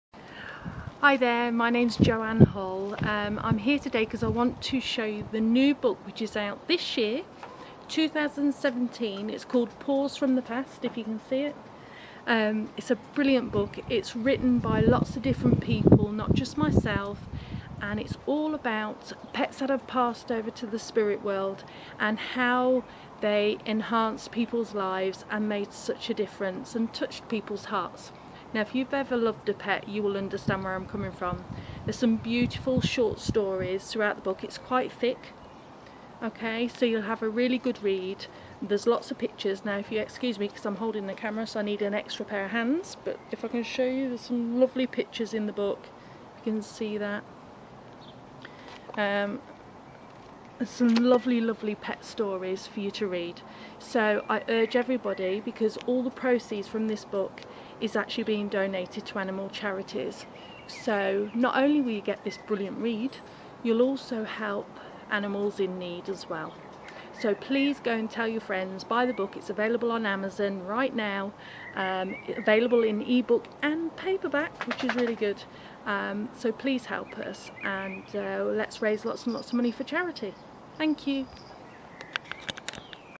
NEW!!! Joanne Hull - Paws from the Past charity book
Joanne Hull the UK's leading Animal Communicator, Speaker, Animal Advocate and Vegan, donates ALL her royalties from this new book to animal charities. Available from Amazon worldwide. Featuring true stories from animal mums and dads about love, life and loss, get your tissues ready, it is full of wonderful moments that melt your heart! AND. It has a 5 star rating! Amust read for anyone who loves animals :) x